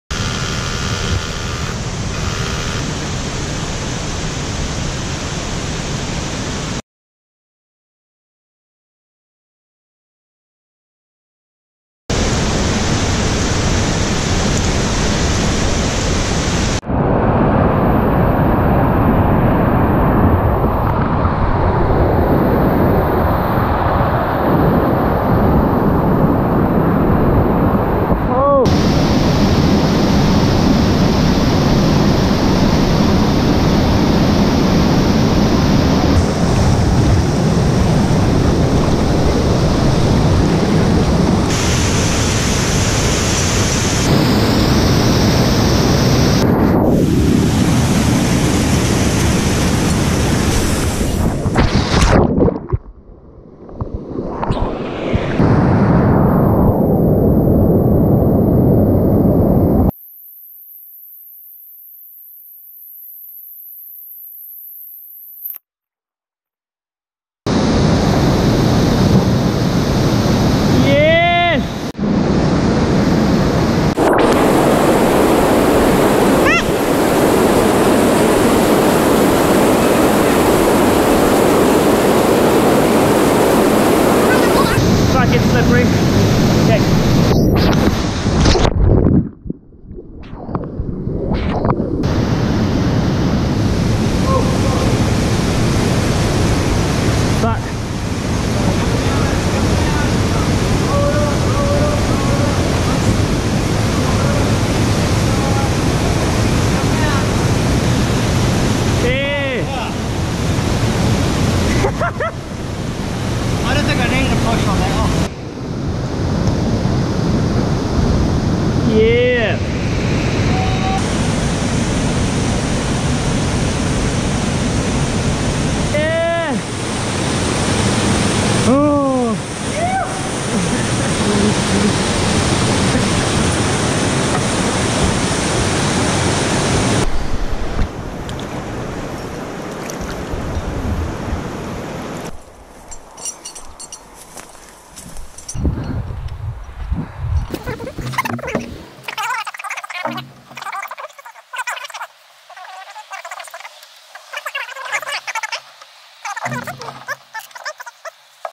Canyonaking Cameron Creek NZ
Doing some Explooooring. Gettin sweaty in an deep creek.